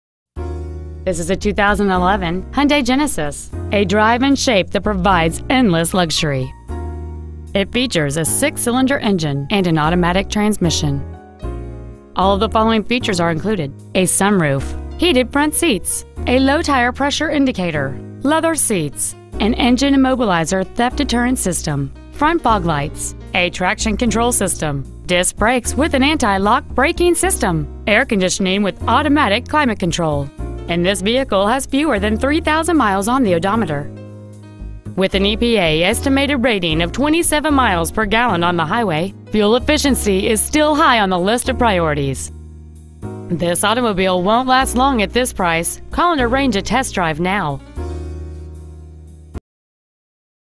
Used 2011 HYUNDAI GENESIS Miami FL
Year : 2011
 Make : HYUNDAI
 Model : GENESIS
 Engine : dohc cvvt 24v v6
 Trans . : automatic
 Exterior : silver
 Miles : 2,985
 Interior : silver

 Lehman Mazda